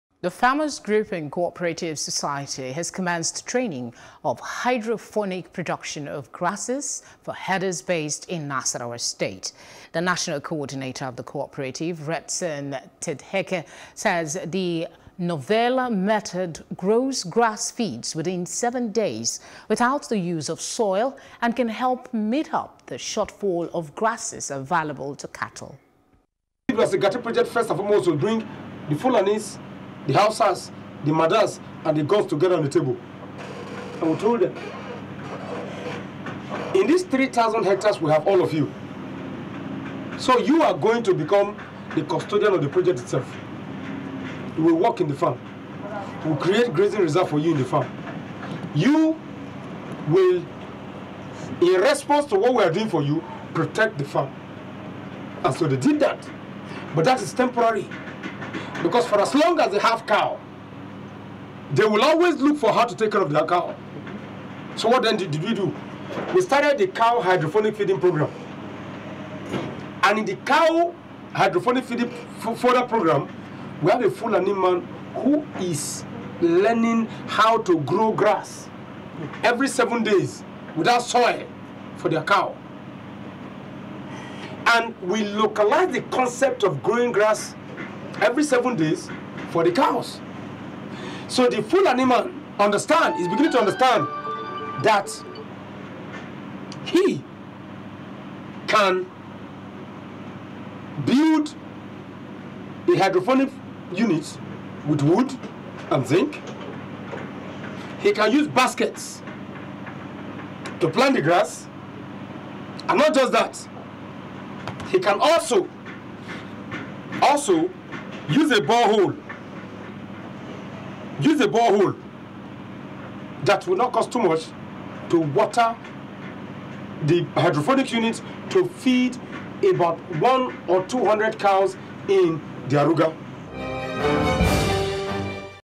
Farmers/Herders Clashes: Farmers Introduce Cow Hydroponic Feeding Prog
The Farmers Group and Cooperative Society has commenced training of hydrophonic production of grasses for herders based in Nasarawa state.